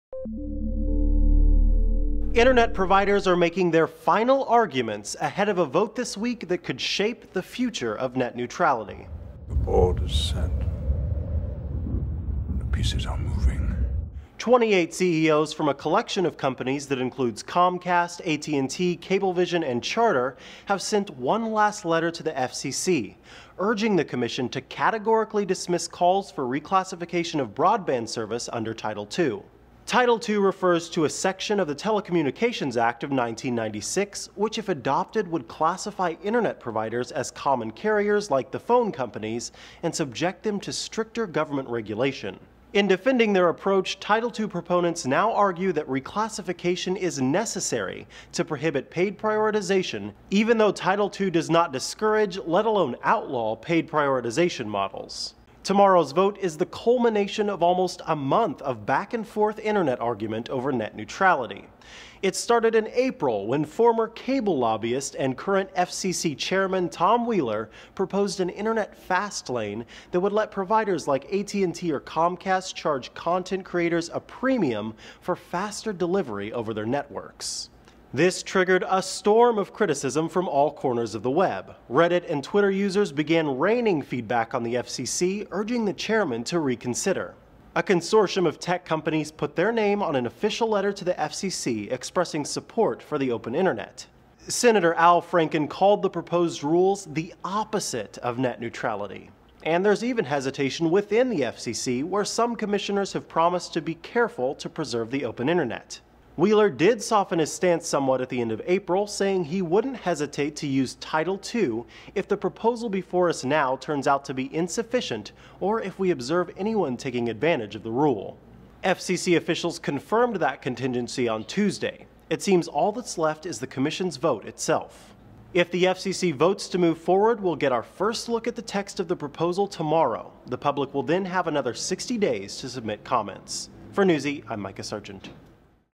Internet Providers Make Last Stand On Net Neutrality Vote
Internet providers say calls for classifying their services under Title II of the 1996 Telecommunications Act wouldn't solve anything.